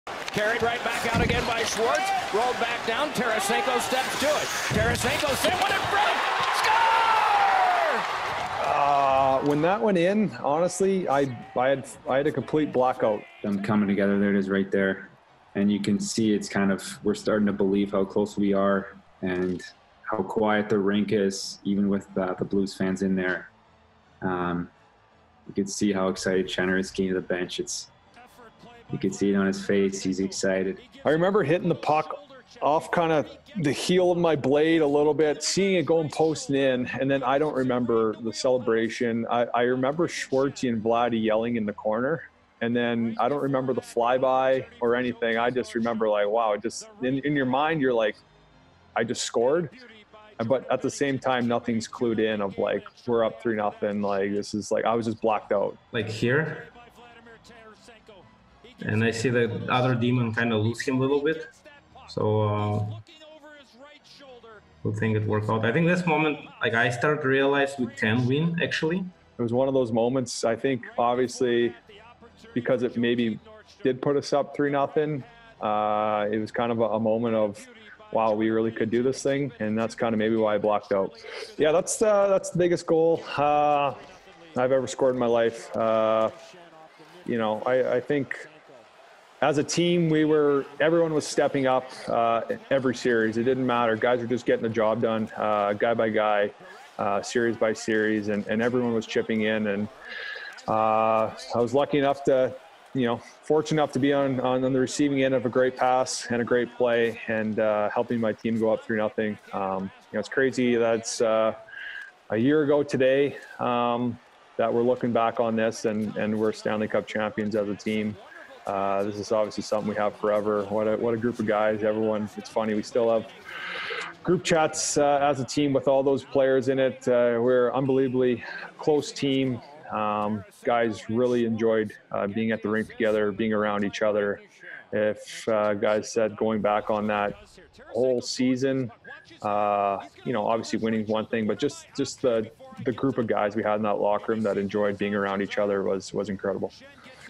Schenn scores in third period
Brayden Schenn says his third period goal in Game 7 of the Stanley Cup Final was the "biggest goal of my life"